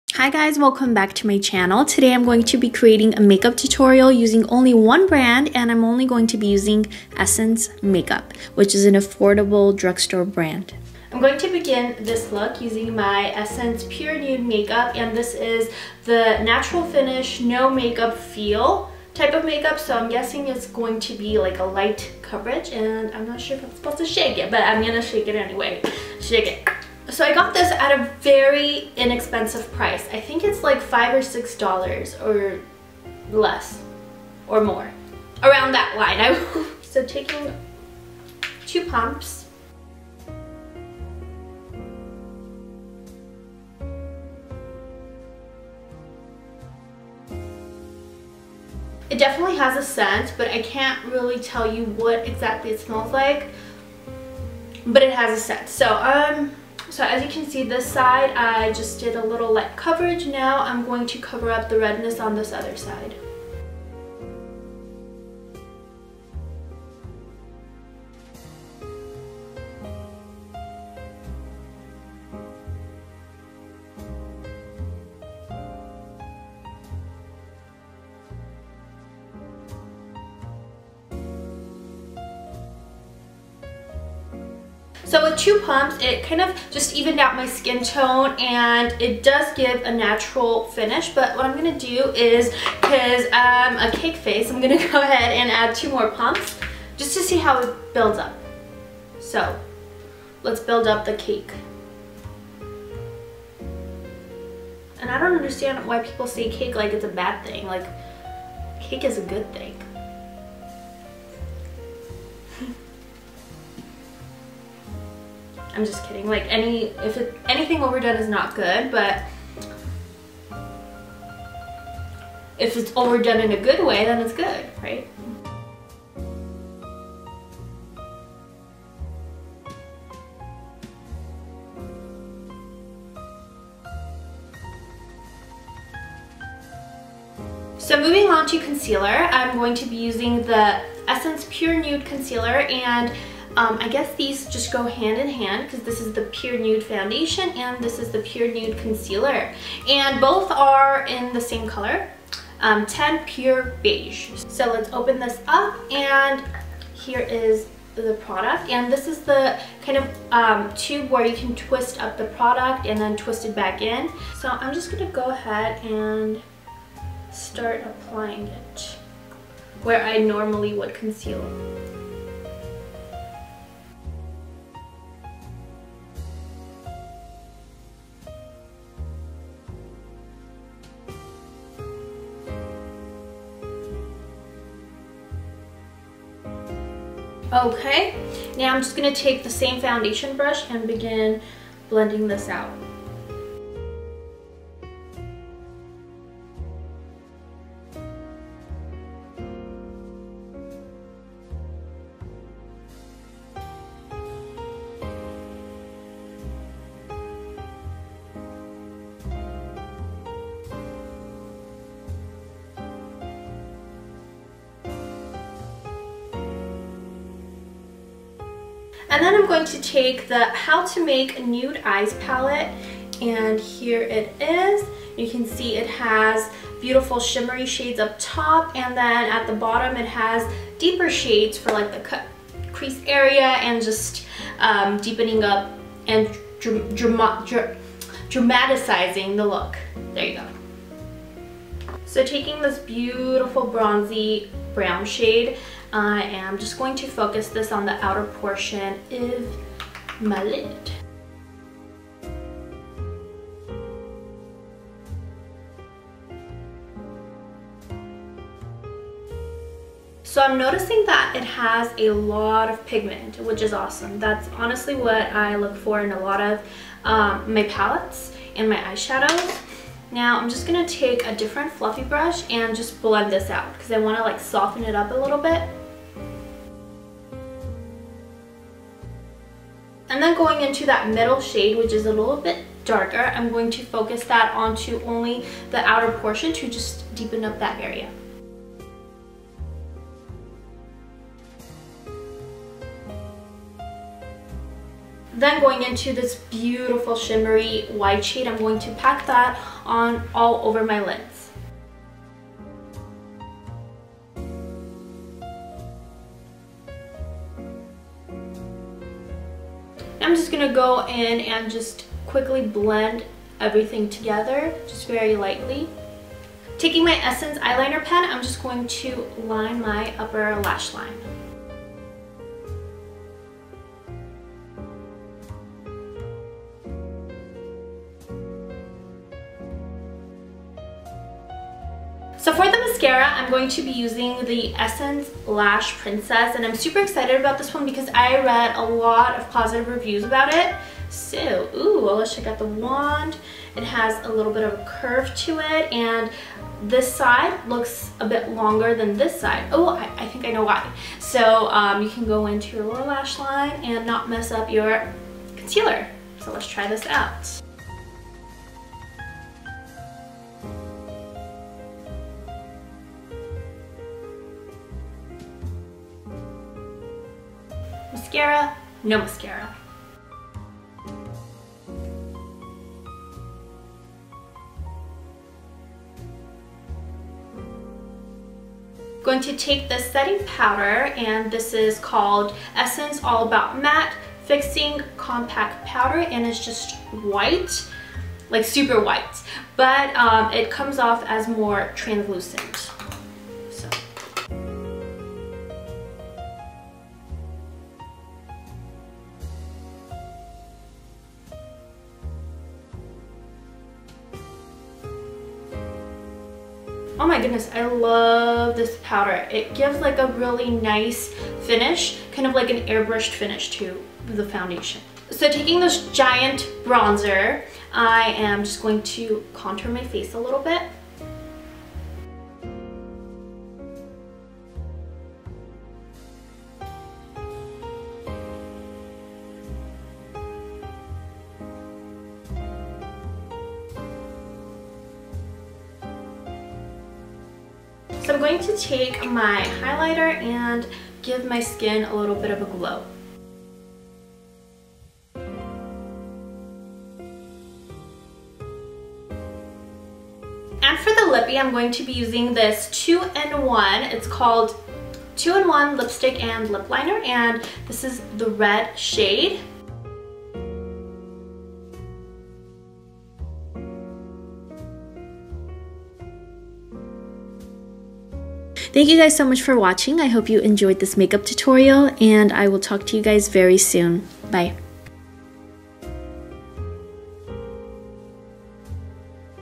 essence | One Brand Makeup Tutorial
Hi loves, here's an affordable 1 brand makeup tutorial using only essence makeup products.  I feel like this look turned out to be a spring glam makeup tutorial lol!

Products used:
essence Pure Nude Foundation
essence Pure Nude Concealer
essence How to Make Nude Eyes Eyeshadow Palette
essence Eyeliner Pen
essence Lash Princess Mascara
essence All About Matt! Fixing Compact Powder
essence Sun Club Matt Bronzing Powder in Blondes
essence Pure Nude Highlighter in Be my Highlight
essence 2 in 1 Lipstick and Liner in Ruby Wow